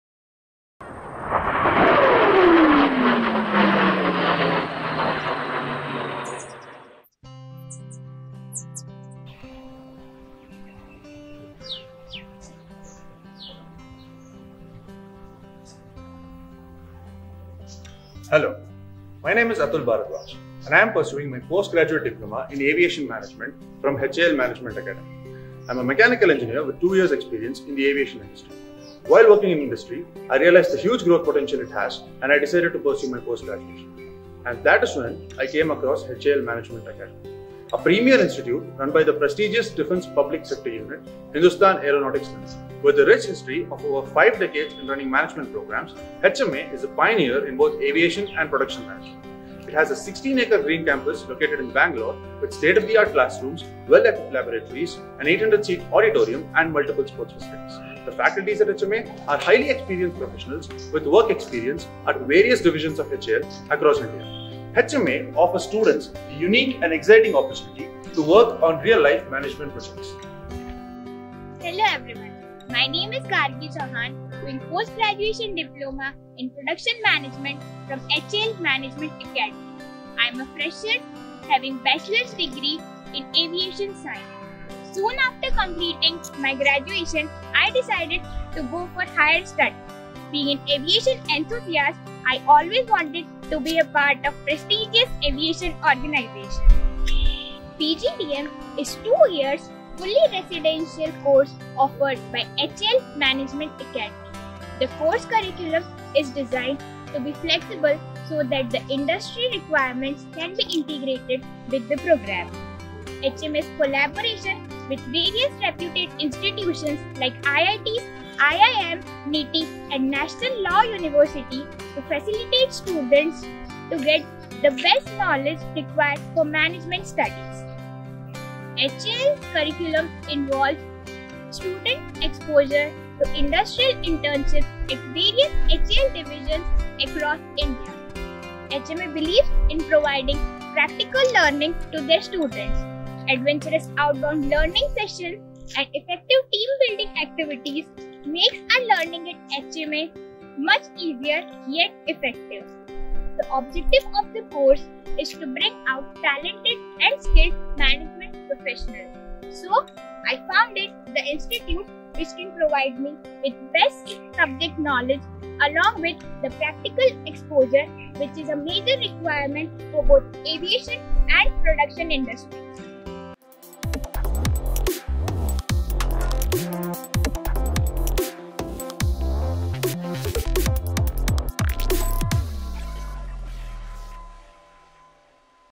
HAL Management Academy - An opportunity to prosper
HAL Management Academy, premier institute of Hindustan Aeronautics Limited is offering PGDM courses in Aviation Management and Production Management. The Indian Aerospace Sector is ready for rapid growth, both in Military and Civil domain. With the growing scope of specialized requirements in Aviation Industry and under the guidance of faculty members with years of working experience, one can have a bright future in this field.